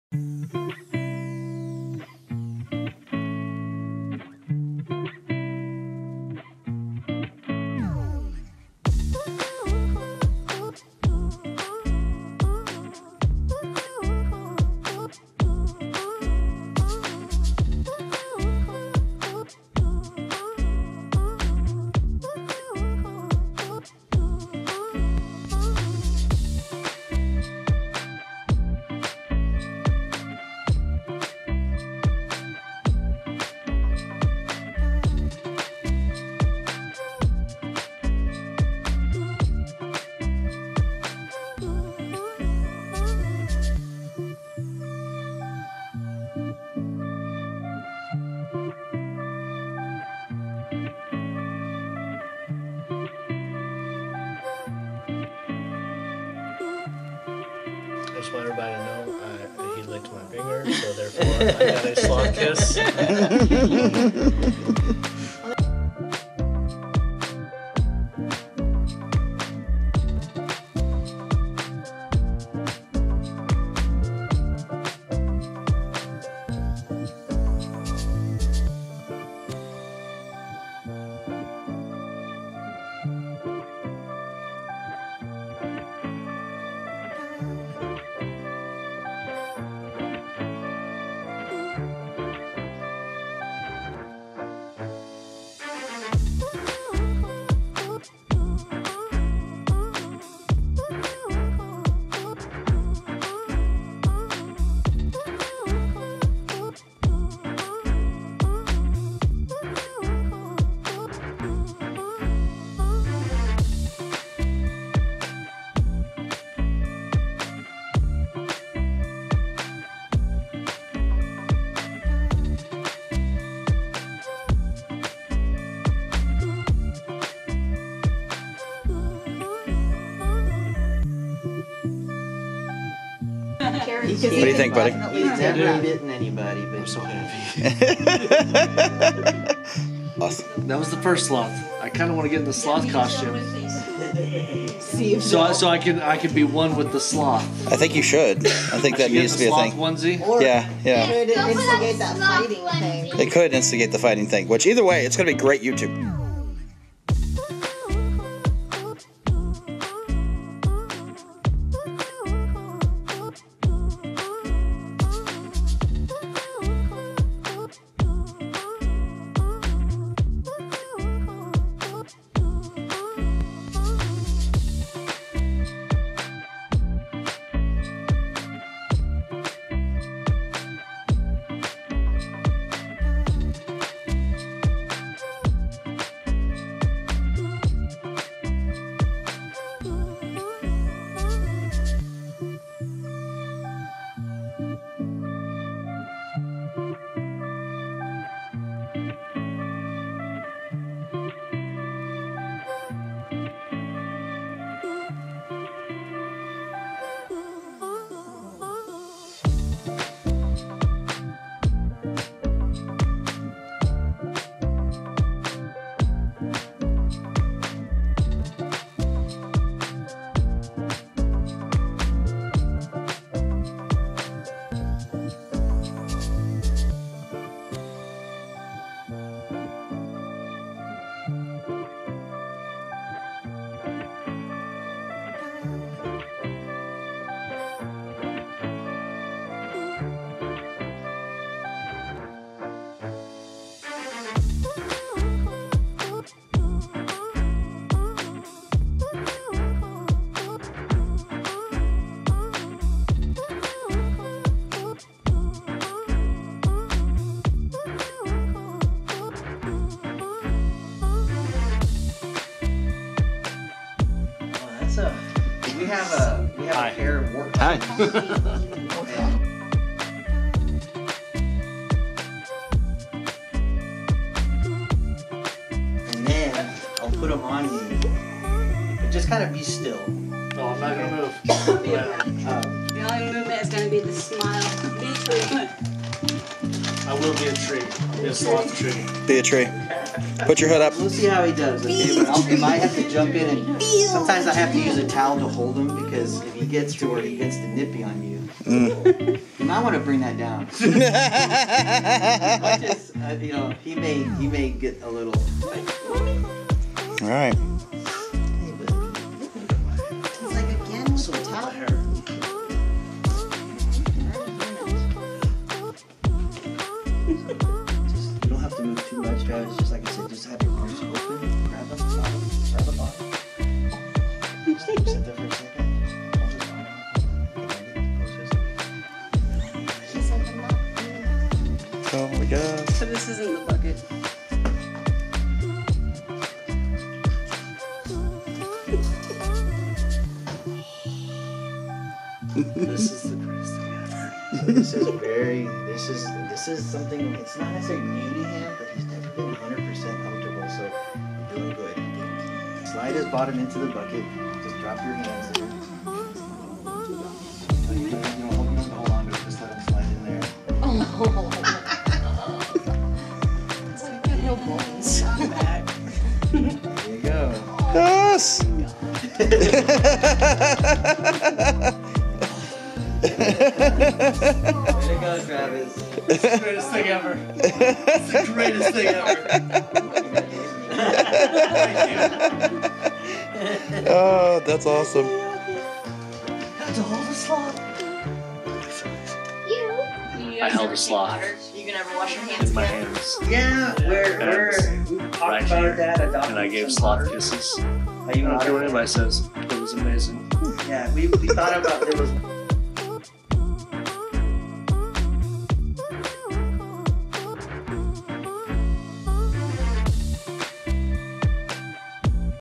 Groovy's Super Sloth Birthday
Most awesome gift I have ever gotten!!! Sloths (my favorite animal EVER) brought to my house and getting to have my family and friends there to experience with me was icing on the cake. Thank you Micah for coming up and recording and making this video for me!